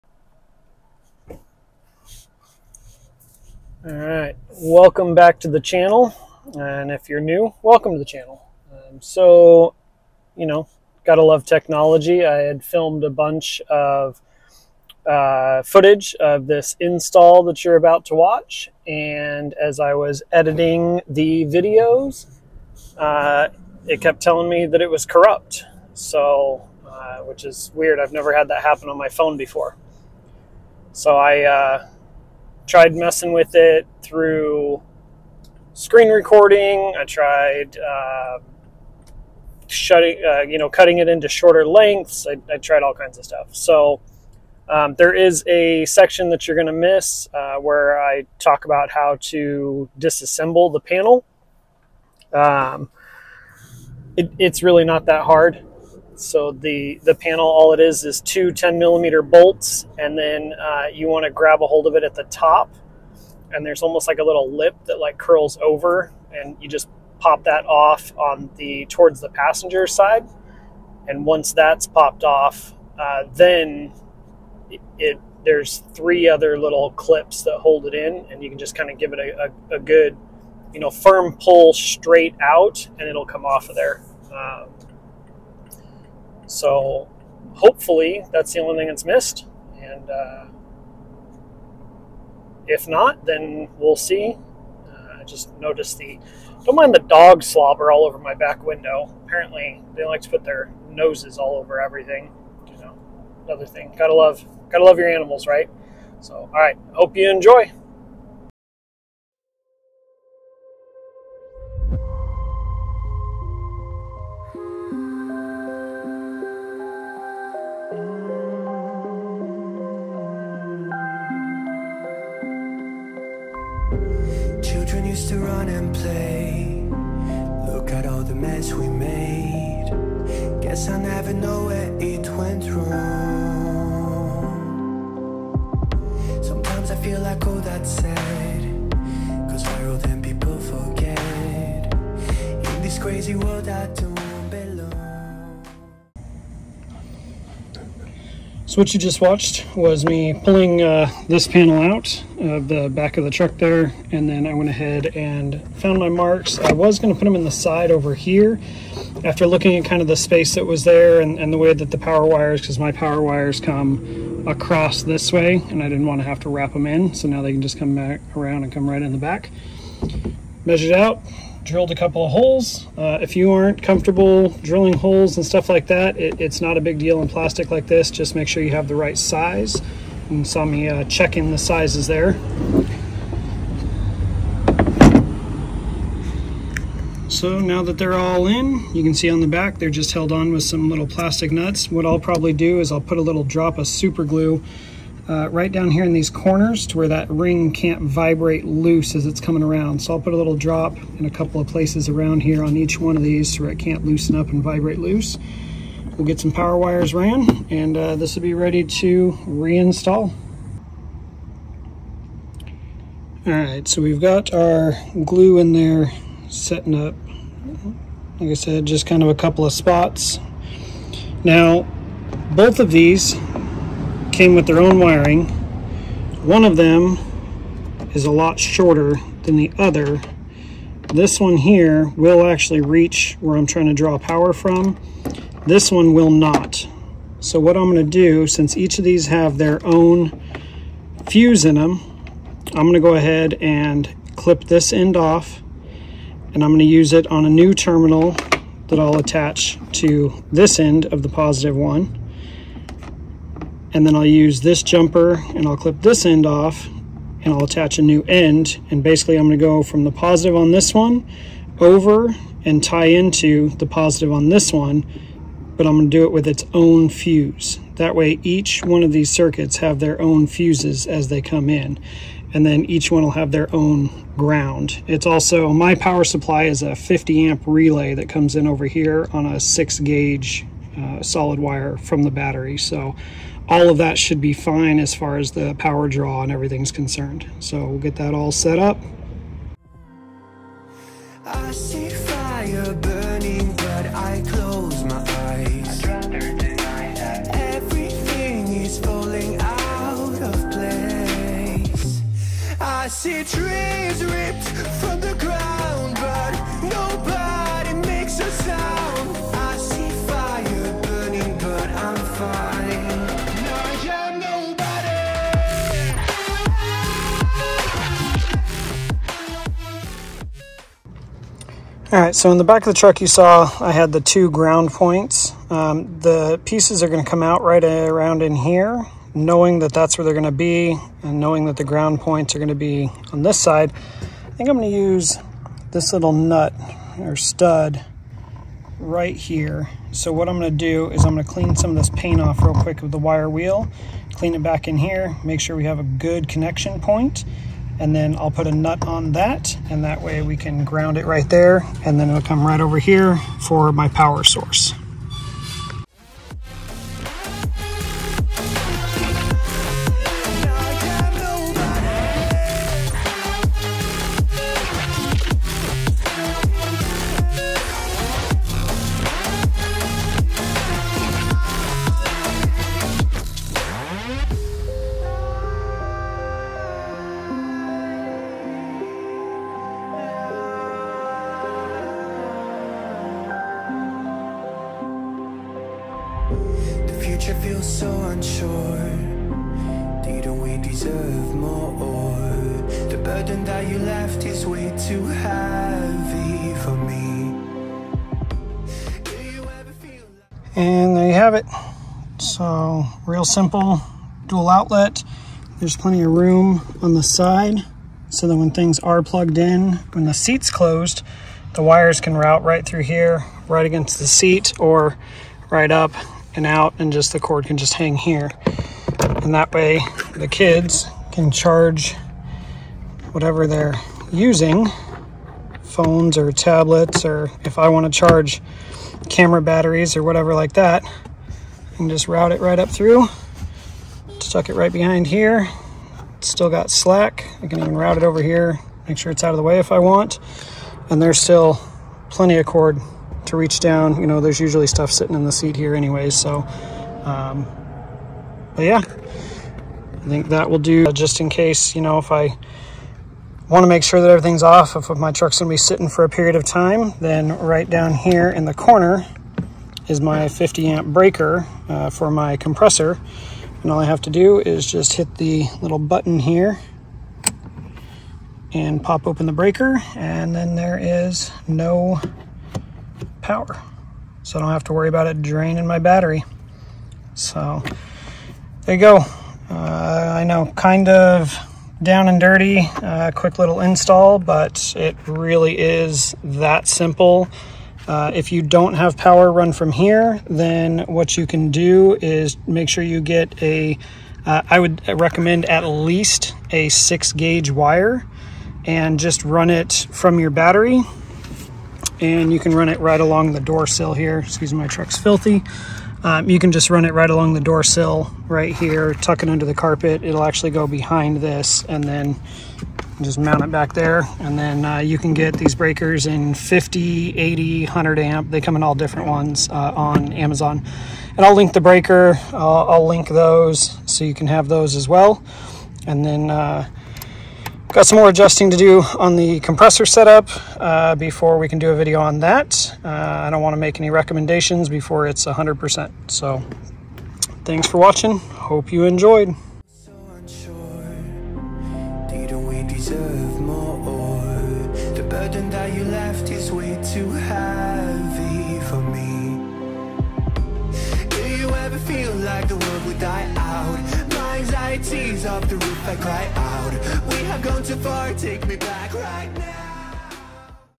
Adding charging ports in the back of a 3rd gen tacoma.
12v circuit breaker:

12v cigarette lighter adapter:

12v dual USB charger: